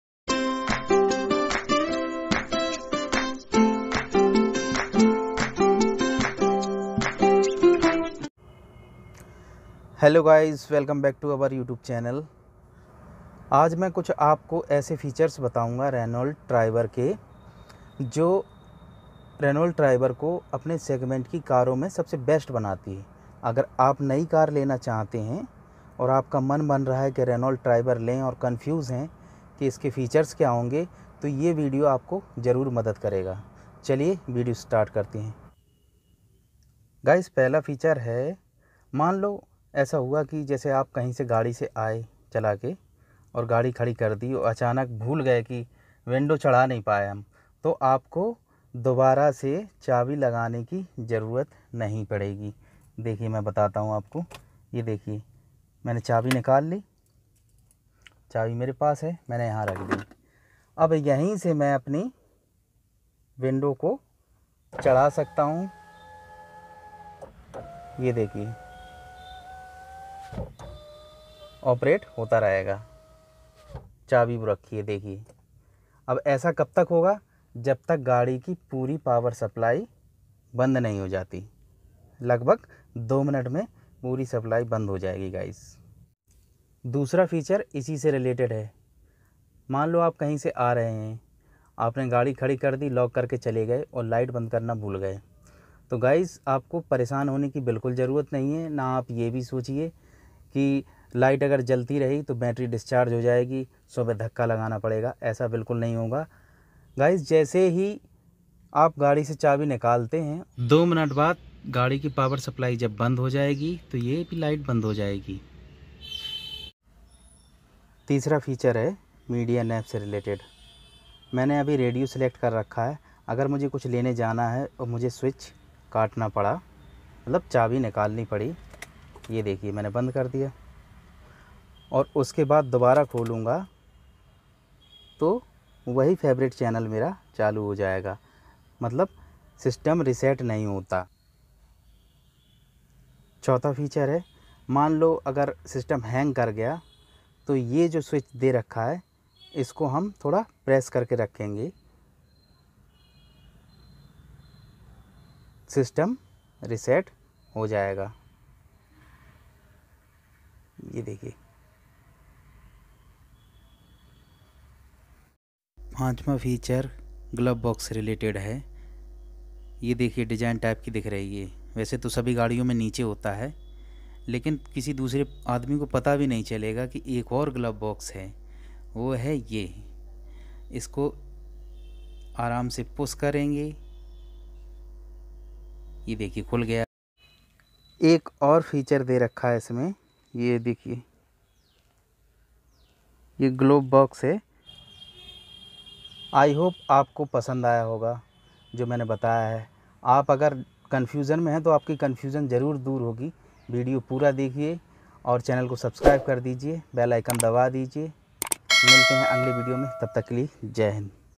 Renault Triber RXT 5 Hidden features🤔||Sècret Features of Renault triber rxt😍 watch till End.
I
 इस वीडियो में मैंने रेनॉल्ट ट्राइबर Rxt variant के 5 Hidden फीचर्स बताए है जो Renault triber को अपने सेगमेंट की सभी कारों में सबसे यूनिक बनाते है